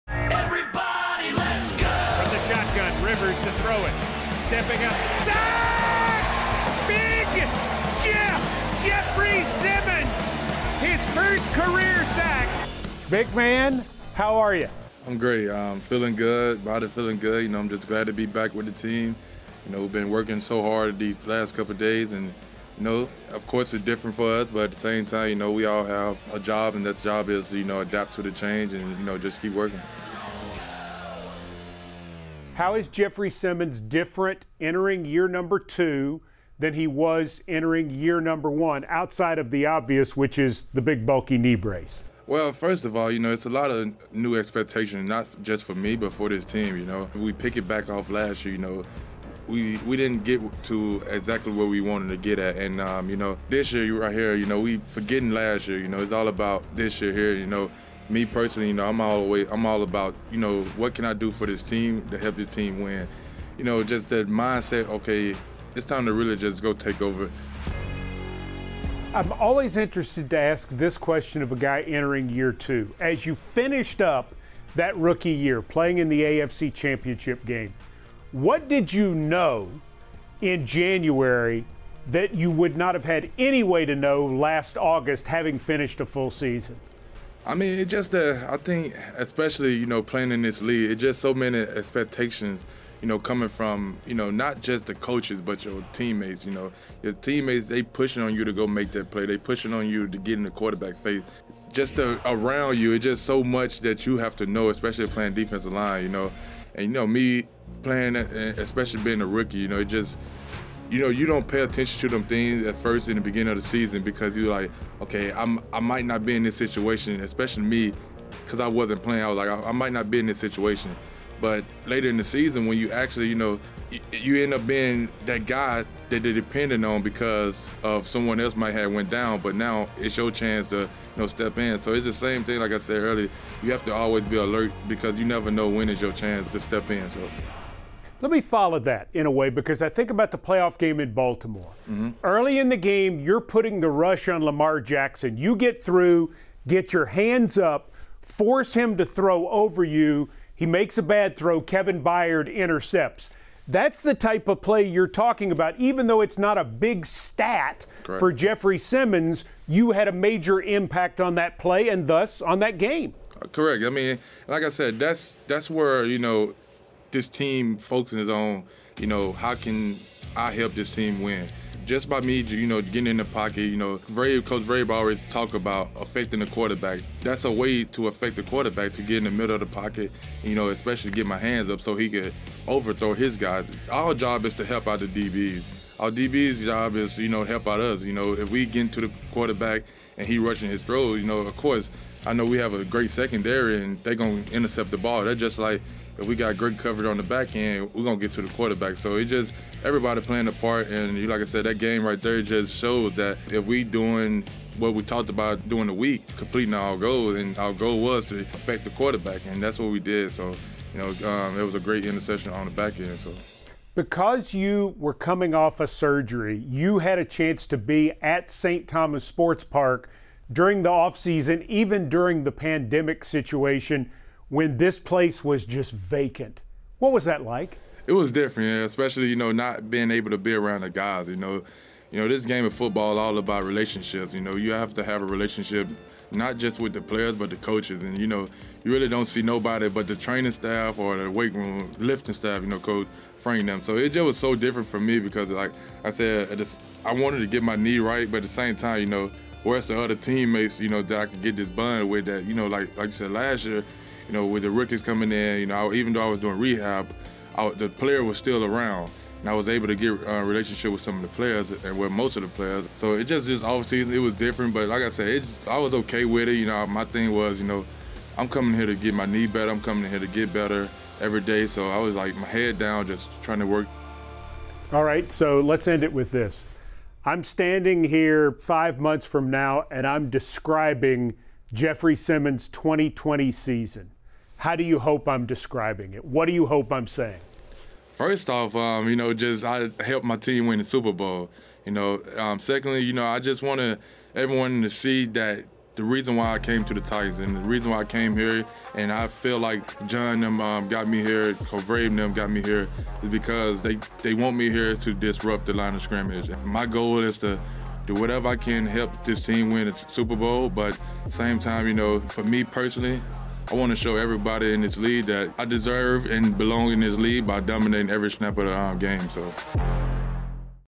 Jeffery Simmons: It's Time to Take Over | Nissan Insider
"Voice of the Titans" Mike Keith visits with second-year defensive lineman Jeffery Simmons, who outlines his goals for the 2020 season, in this Nissan Insider.